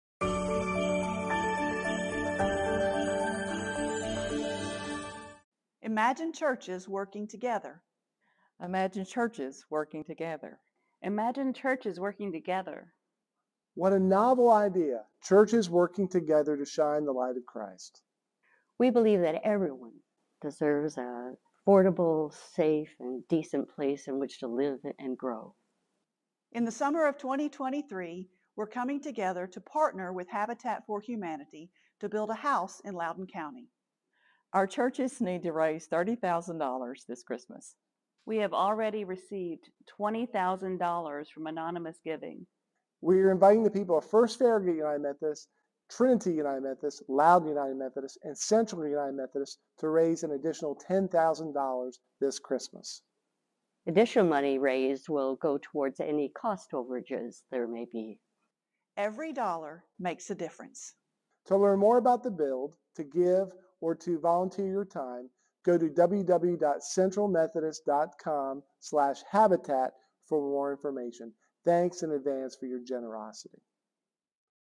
Christmas Mission Offering 2022
We're partnering with Trinity United Methodist, Loudon United Methodist and First Farragut United Methodist Churches this Christmas. for more information.